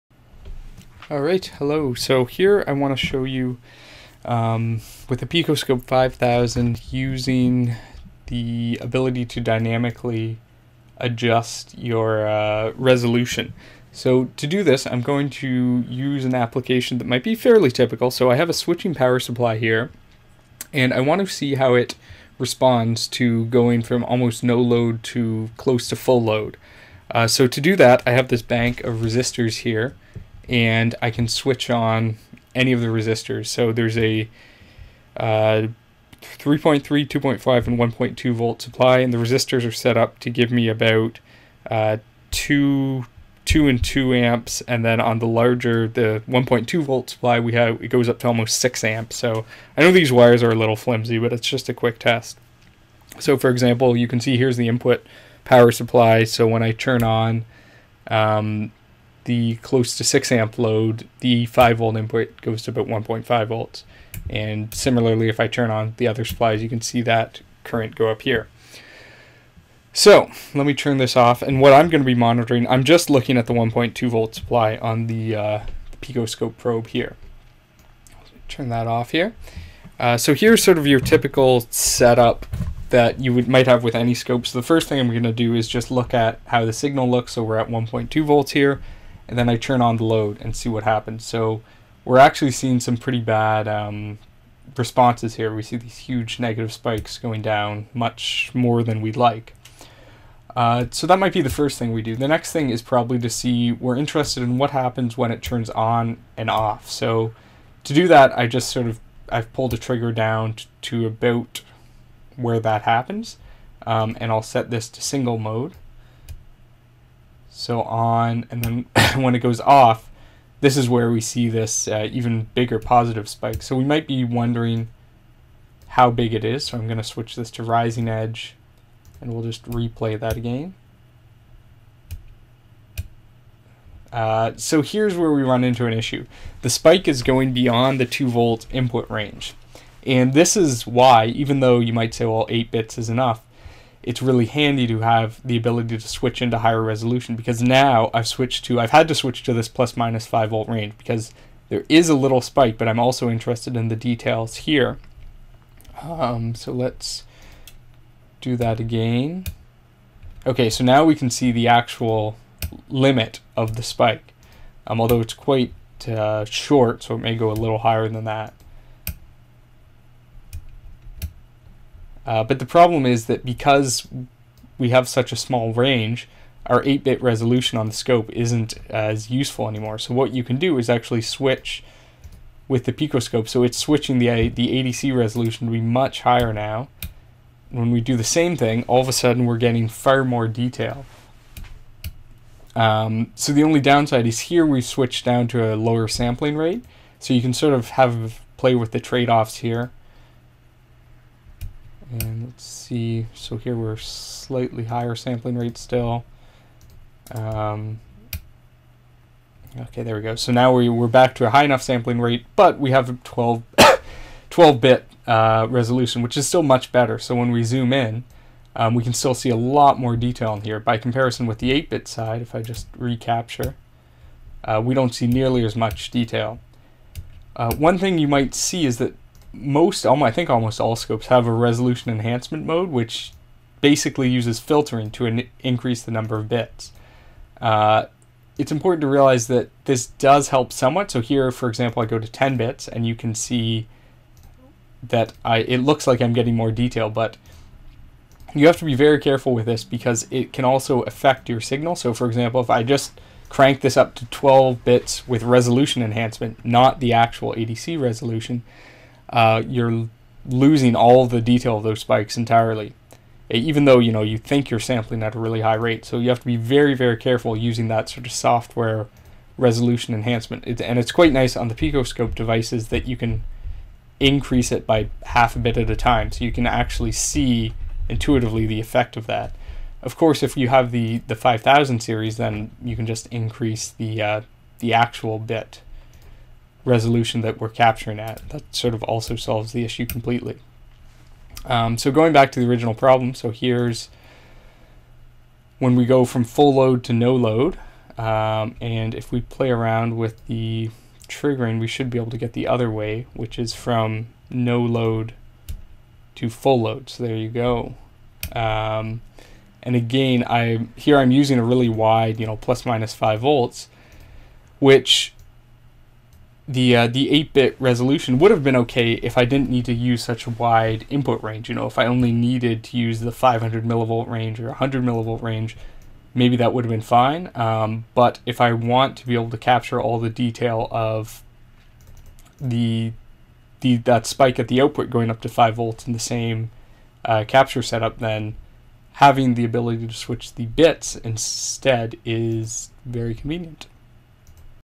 Switching PSU Load Test - 8 vs 12 bit Resolution (PicoScope 5000)
The PicoScope 5000 Series allows you to dynamically change the ADC resolution. If you're trying to get a high dynamic range this is a godsend, as for example happens with a large spike in one part of the trace which you need to capture, but you still want to see detail about the rest of the trace.